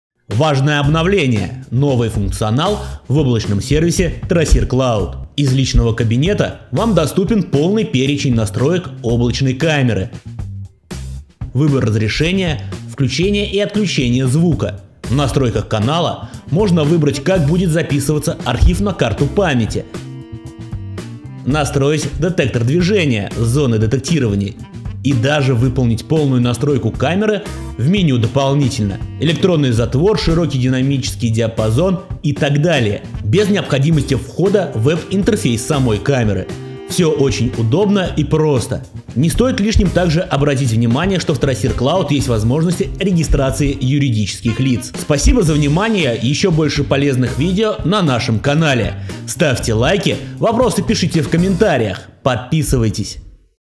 Важное обновление! Не пропусти! Облачный сервис TRASSIR Cloud видеонаблюдение в облаке
Из личного кабинета вам доступен полный перечень настроек облачной камеры,
выбор разрешения, включение и отключения звука.

 В настройках канала можно выбрать как будет записываться архив на карту памяти, настроить детектор движения - зоны детектирования и даже выполнить полную настройку камеры в меню Дополнительно, электронный затвор WDR, и т. д. без необходимости входа в веб интервнйс камеры. Все удобно и очень просто.